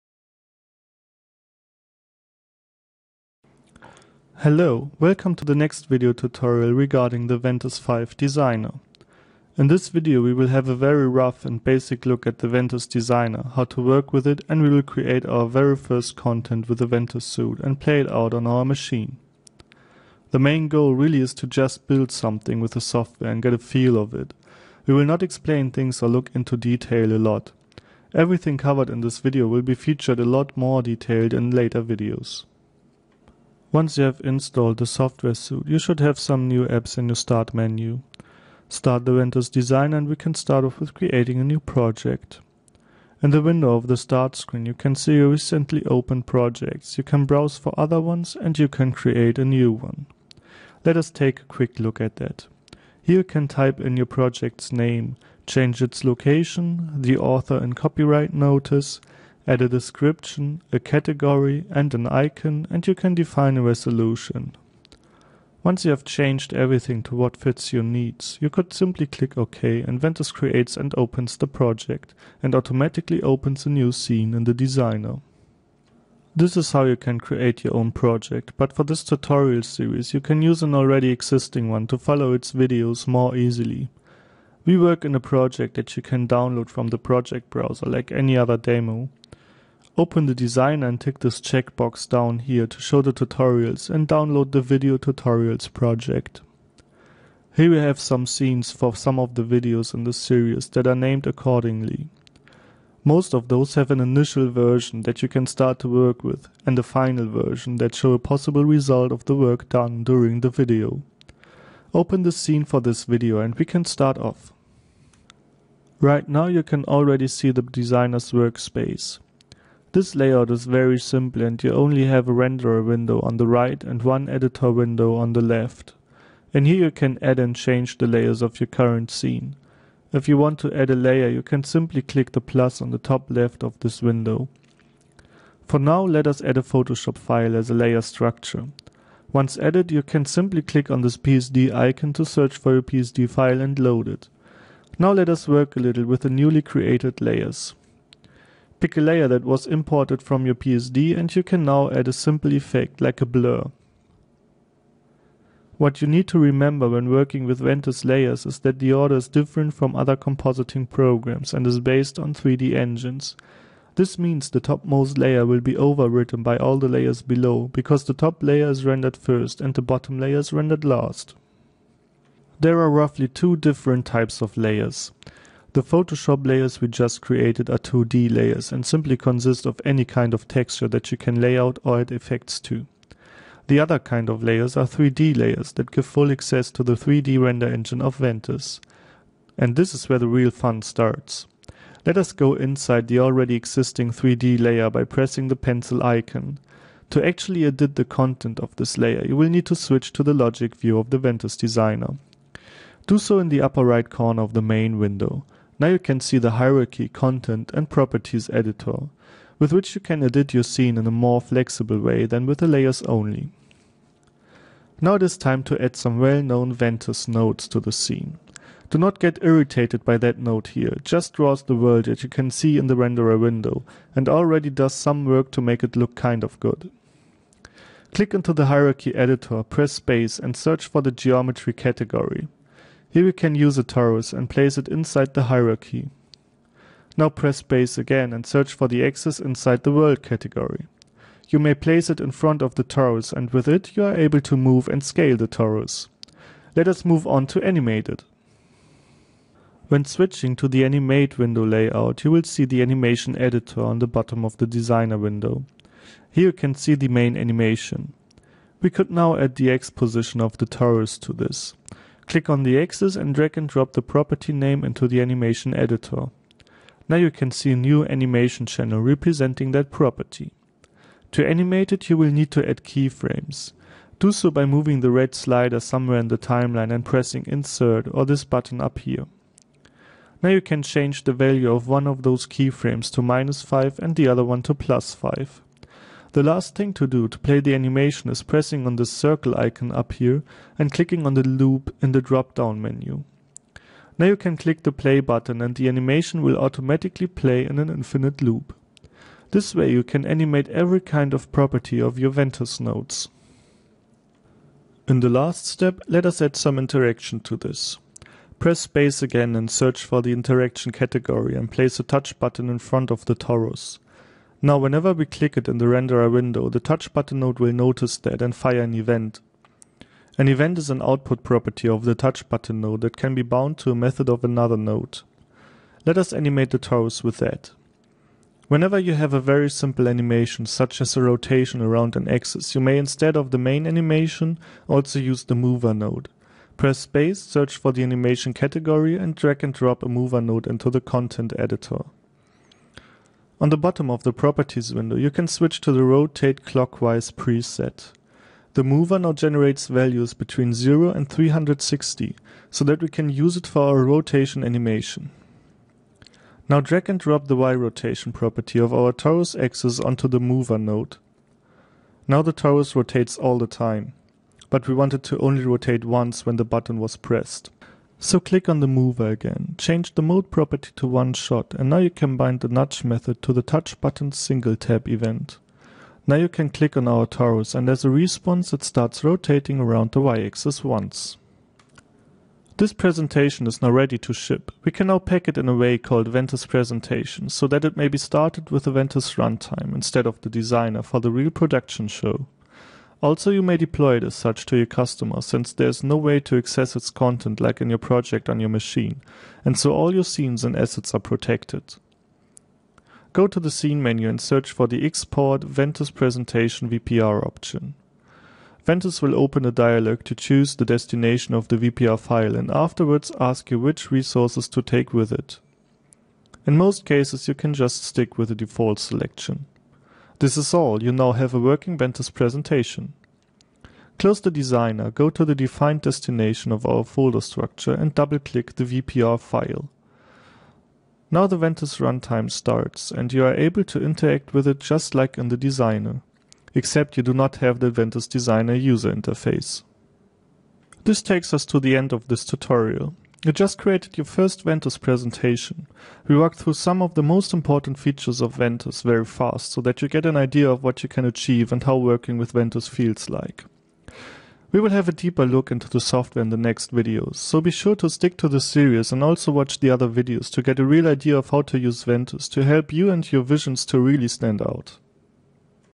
Ventuz 5 Tutorial : 01 Designer Overview
This is a basic introduction to the workflow of the Ventuz Software Suite. You will create your very first Ventuz Presentation and learn the most basic concepts behind the Software.